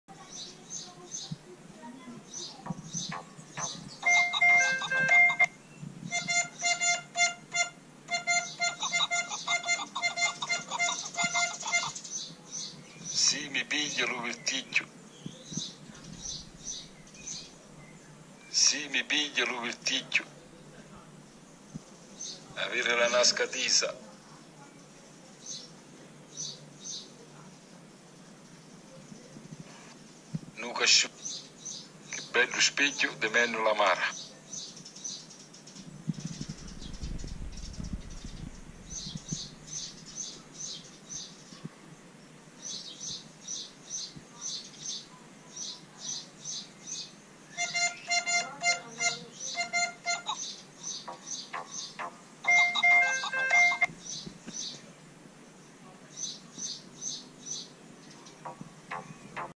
How to use the Sicilian Gestures app
Funny app with 81 gestures of Sicily; listen to a local Sicilian speaker; translated into six different languages: Italian, German, 中文 (Chinese), English, Turkish and Spanish!

Enjoy and learn the secrets of the famous Sicilian body language!